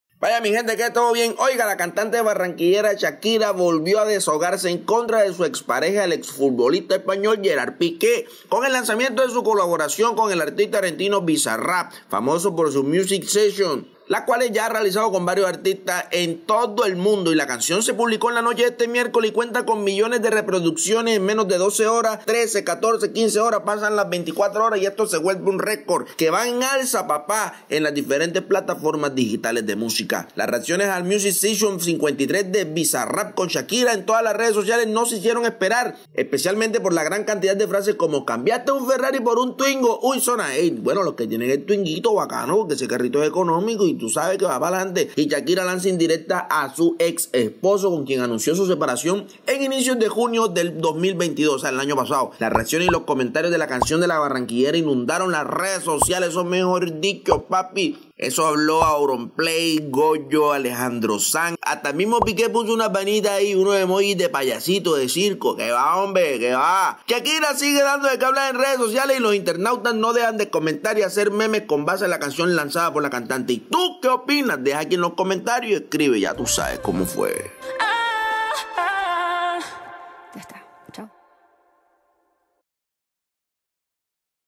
La cantante barranquillera, Shakira, BizaRap
GsEntretenimiento | La cantante barranquillera, Shakira volvió a desahogarse en contra de su expareja, el futbolista español Gerard Piqué, con el lanzamiento de su colaboración con el artista argentino Bizarrap, famoso por sus Music Session, las cuales ha realizado con diferentes artistas de todo el mundo.